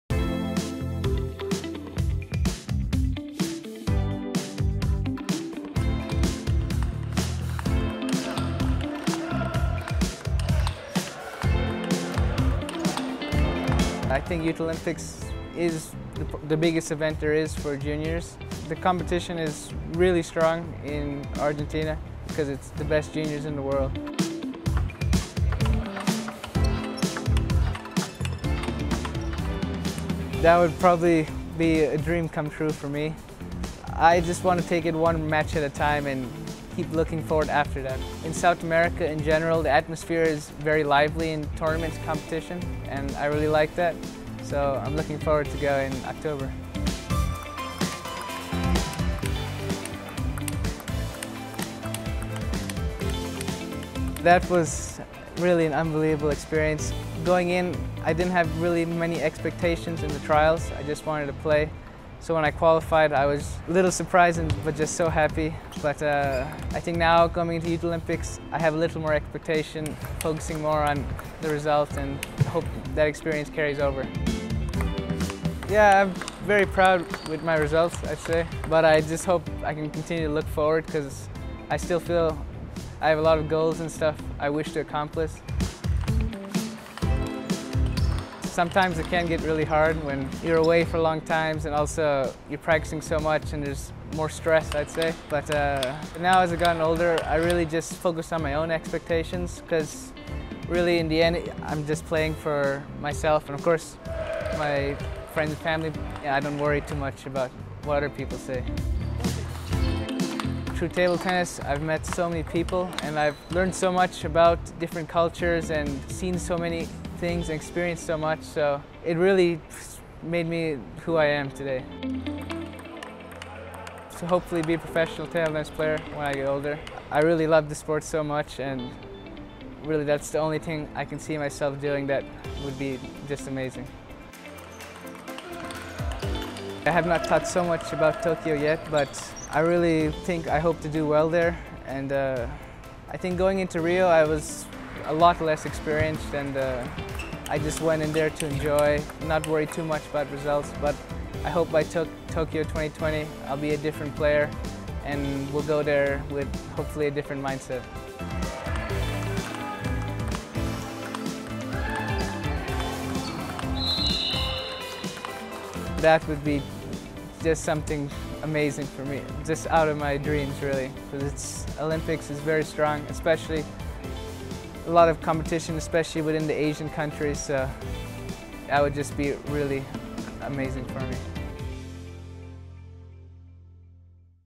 Kanak Jha: USA's newest Table Tennis Talent | Youth Olympic Games
Being only 16-years-old, Kanak Jha was the youngest team member of the USA at the Olympic Games in Rio. Kanak has set his sights on gold at this year's Youth Olympic Games in Buenos Aires.